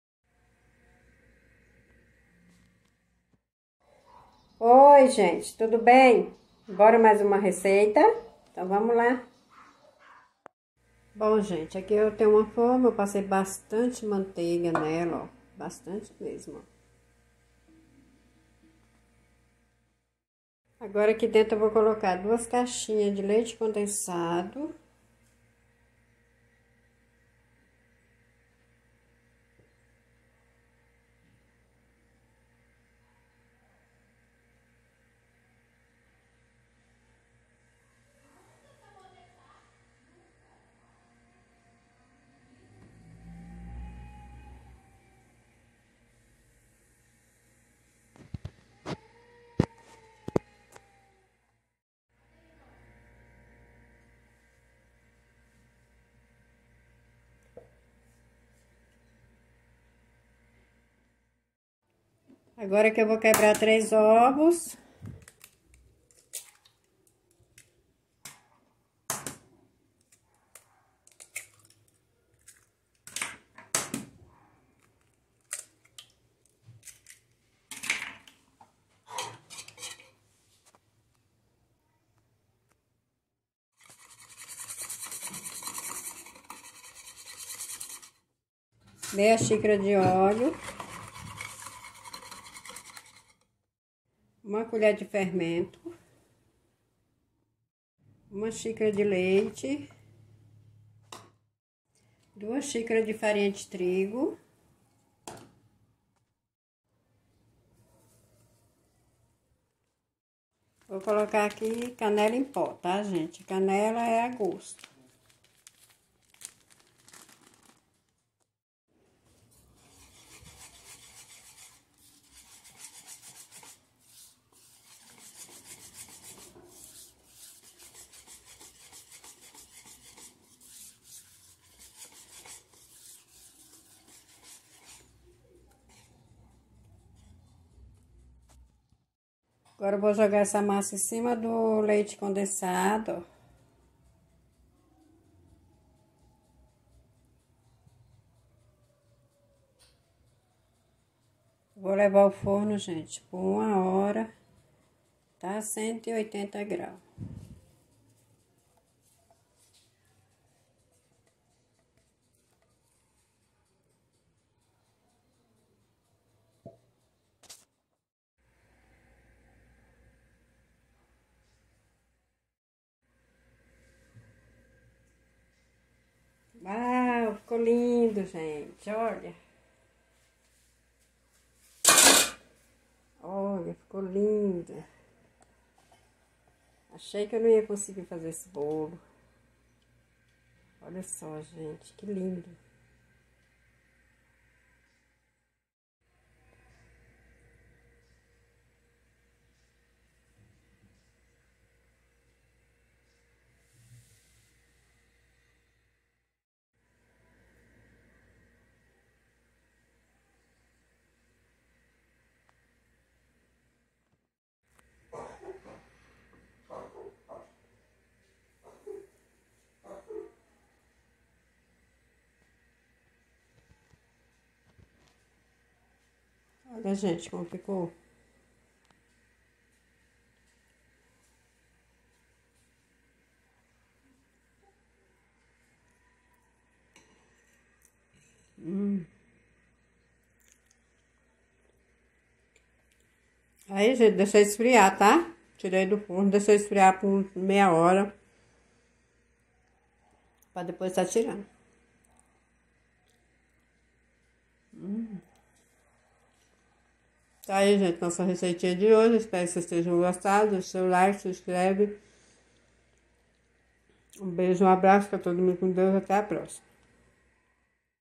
🙏 VC VAI QUERER COMER ISSO HOJE😋🥰🥰🥰🥰🥰🥰🥰♥️♥️♥️
2 caixinha de leite condensado
3 ovos
2 xícara de farinha de trigo240g
1xicara de leite240ml
1 colher de fermento em pó
1/2 xícara de óleo
1 colher rasa  de canela
1 xícara de açúcar 240g

caixa postal:7827.
cep:72305970.